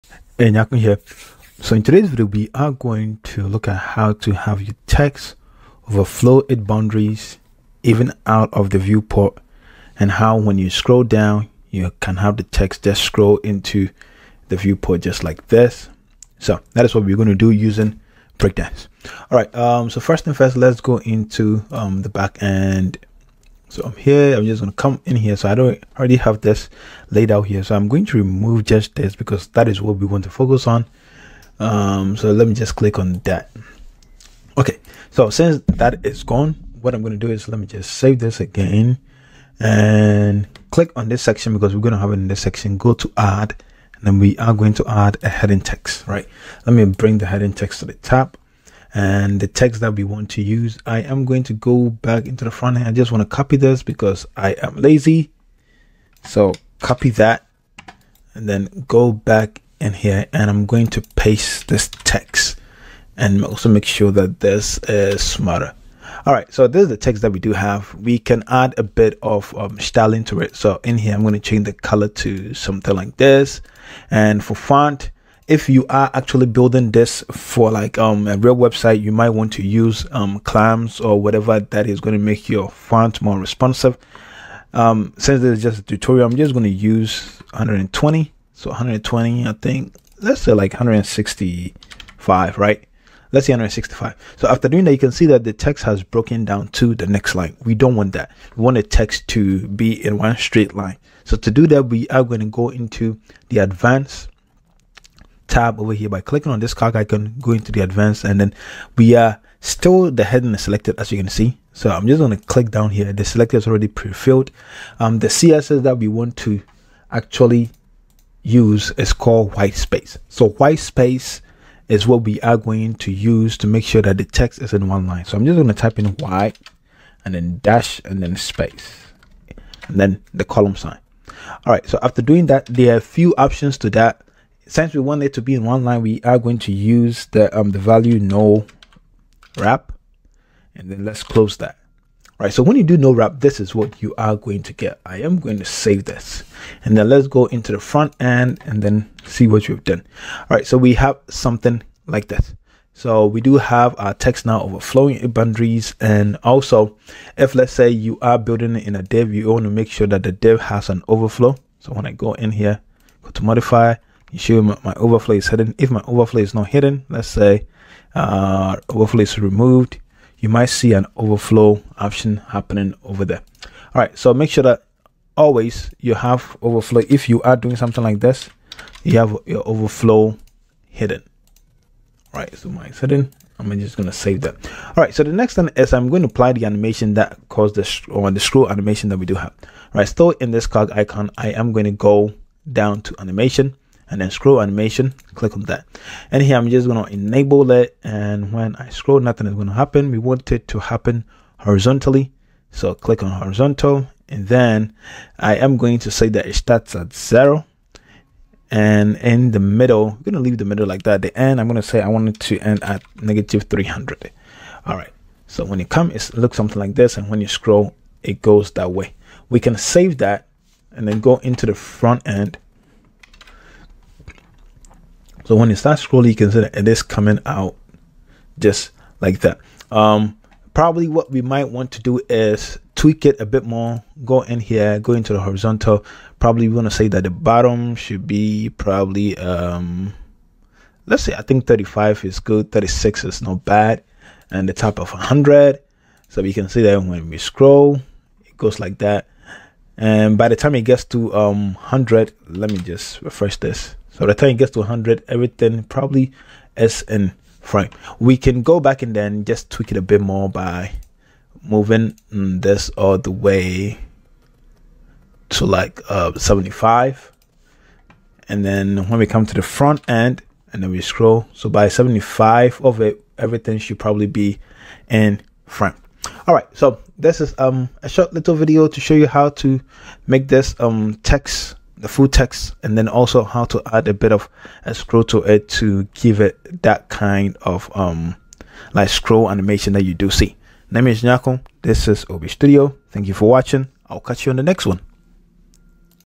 Overflowing Text with Scroll Animation using Breakdance
How to build an overflowing text and scroll trigger animation using Breakdance Builder.

🕒 Timestamps:
00:00 - Introduction
00:21 - Getting Started
00:39 - Building The Overflowing Text
01:34 - Adding CSS styling
03:04 - Applying The Animation
04:37 - Conclusion: Recap and Final Thoughts